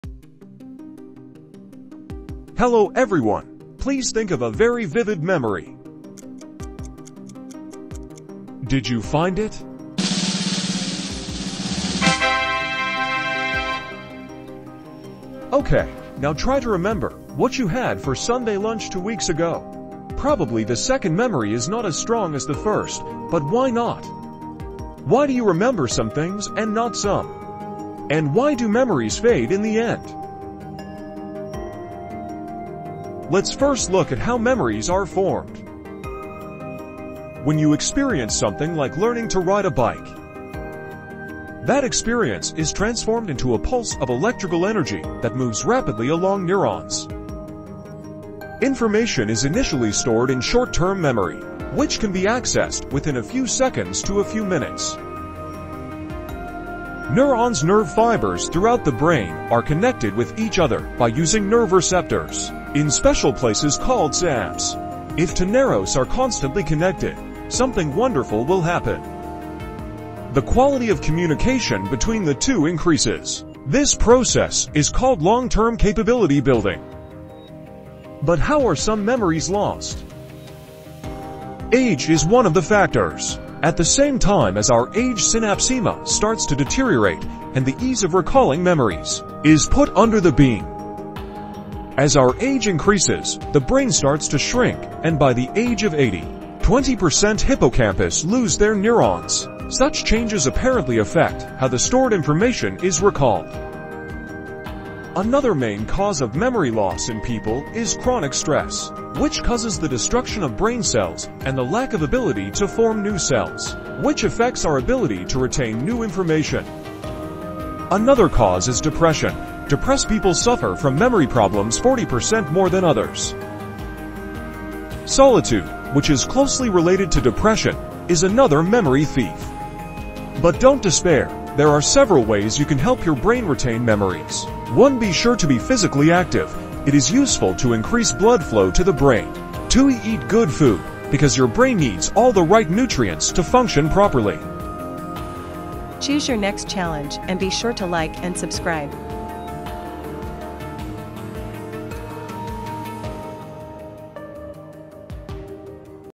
How do we lose memories?
How do we lose memories? It's a peculiar yet universal phenomenon that many of us experience throughout our lives. Memories, those fragments of our existence that stitch together our past, can sometimes slip away, leaving us grasping at recollections that seem to fade with time. The contrast between vivid, impactful memories and the gradual erosion of seemingly inconsequential ones prompts us to ponder: Why do memories evade our grasp, and what causes their eventual fading?

Consider a vivid memory. Perhaps it's the exhilarating feeling of accomplishment when you mastered riding a bike, a memory etched vividly in your mind. The sheer excitement and novelty of that experience might seem ingrained, almost effortlessly recalled at will. Yet, try recalling a mundane detail from a few weeks ago, such as the contents of a Sunday lunch. The struggle to recollect with the same clarity highlights the selective nature of memory.

The formation of memories is a complex process orchestrated by the intricate dance of neurons within the brain. Experiences, like learning to ride a bike, trigger electrical impulses that cascade through neurons. Initially stored in short-term memory, these experiences transition to long-term memory via areas like the hippocampus. The quality of communication between neurons, known as long-term potentiation, solidifies these memories. Yet, despite this intricate process, memories can dissipate over time due to various factors.

Age is a significant contributor to memory loss. As we age, synaptic connections weaken, impacting our ability to recall information. The natural aging process sees a reduction in brain size and neuron loss, particularly in the hippocampus, affecting how stored information is remembered.

Moreover, chronic stress exerts a detrimental effect on memory. It leads to the destruction of brain cells and impairs the formation of new ones, hindering our capacity to retain new information. Depression, often linked to memory issues, affects individuals significantly, resulting in a 40% higher prevalence of memory problems than in others. Solitude, intertwined with depression, acts as another memory thief, contributing to memory lapses.

However, there's hope amidst these challenges. Several strategies can assist in preserving and enhancing memory. Physical activity promotes increased blood flow to the brain, aiding its function. A well-balanced diet provides essential nutrients crucial for optimal brain performance. Engaging the brain through activities like learning a new language stimulates neural pathways, preserving cognitive abilities.

In essence, while the loss of memories may seem inevitable due to various factors like aging, stress, depression, and isolation, proactive steps can mitigate these effects. Nurturing both mental and physical health lays the foundation for maintaining and enriching the tapestry of memories that shape our lives. By understanding how memories form and the factors influencing their retention, we can strive to cherish and preserve the invaluable mosaic of our life experiences.